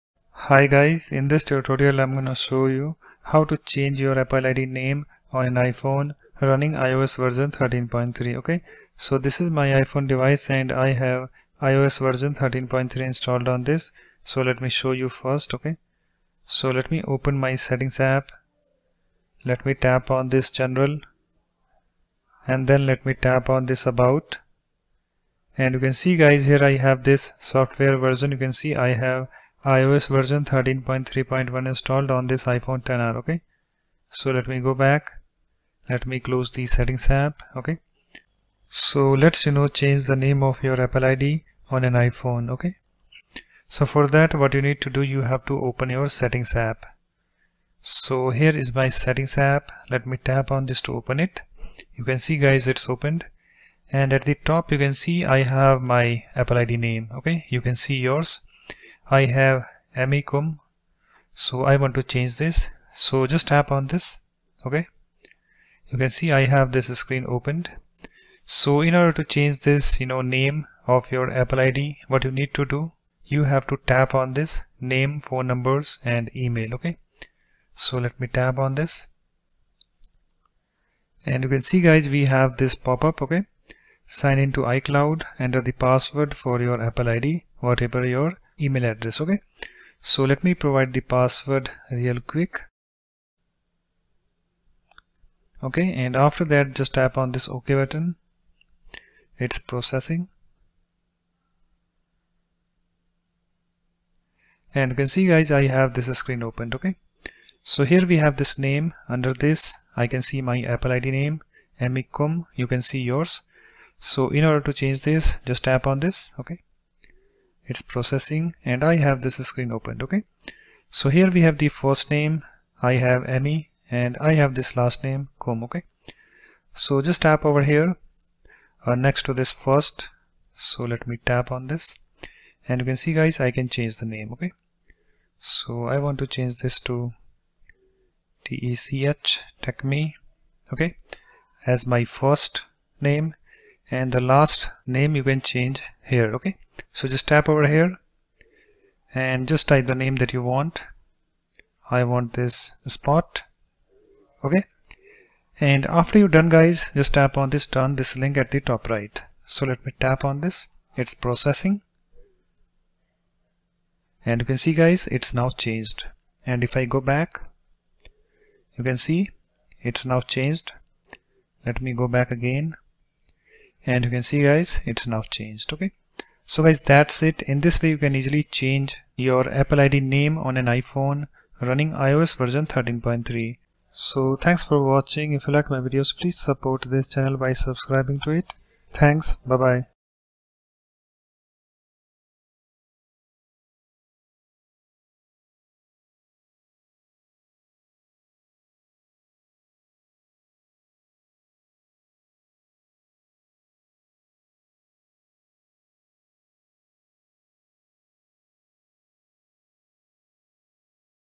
How to Change your Apple ID Name on iPhone (iOS 13.3)?
Step 1: Open 'Settings' App.

Step 2: Tap on your 'Apple ID' Name at the top.

        Note: Sign-in to your Account if you have not done that.

Step 3: Tap on 'Name, Phone Numbers, Email.'

Step 4: Provide the password of you account, and then tap on 'Ok' button.

Step 5: Tap on your 'Apple ID Name' at the top under 'Name' Section.

Step 6: Change the name that you want, and then tap on 'Done' link at the top right.

It's done.